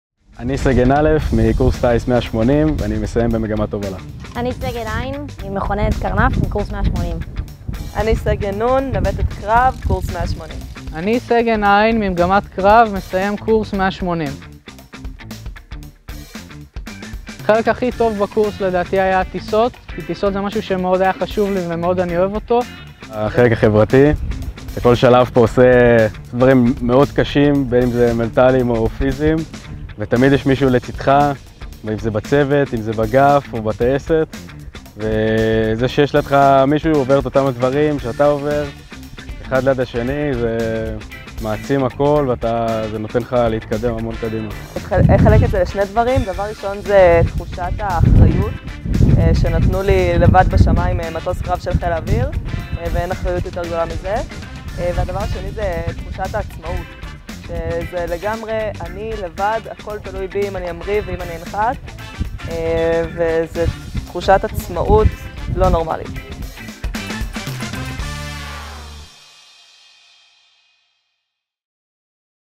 חיל-האוויר הישראלי | רגעי השיא בקורס-טיס
מה היה החלק הכי טוב בקורס-טיס?
מסיימים קורס-טייס 180 עונים!